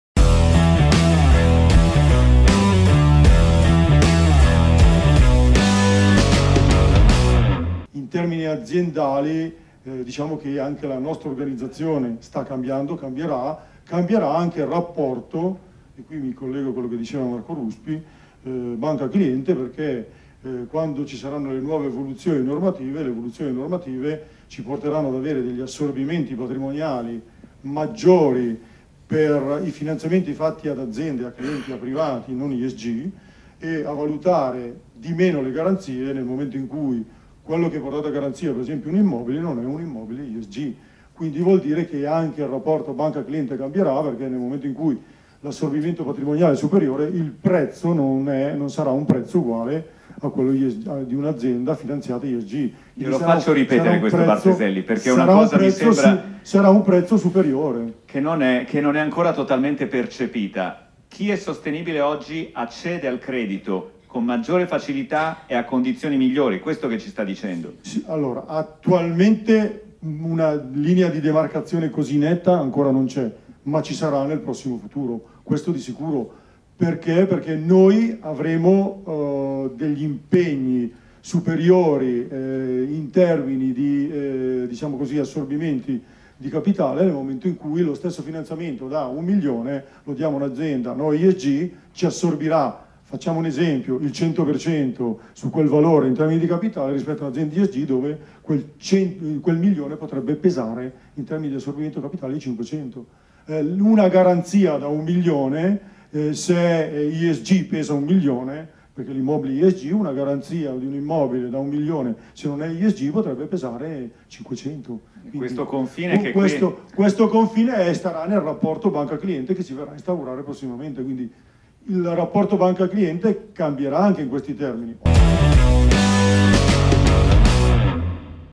Sostenibilità: come cambia il rapporto tra banche e imprenditori
Enrico Barteselli, Vice Direttore Generale di BCC Milano,  ci spiega perché la sostenibilità è un fattore che impatterà sempre di più nel rapporto tra imprenditori e  banche.

“Il percorso per integrare una strategia d’impresa sostenibile” è il titolo della tavola rotonda organizzata da AcomeA SGR e V-FINANCE, tenutasi il 19 aprile presso l’auditorium di BCC Milano a Carugate, a cui ha partecipato una platea di imprenditori locali.